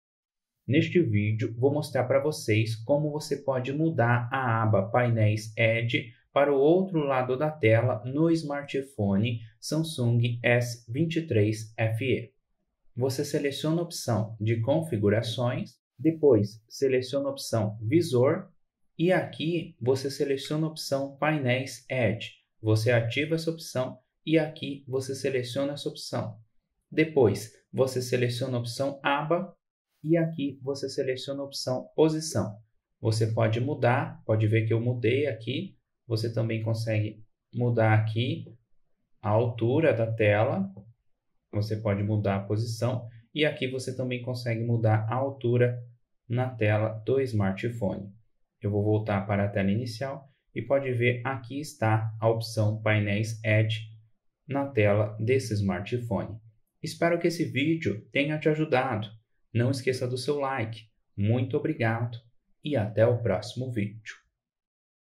Como mudar a aba do Painel Edge para o outro lado da tela no smartphone Samsung S23 FE
Neste vídeo vou te ensinar como mudar a aba do Painel Edge para o outro lado da tela no smartphone Samsung S23 FE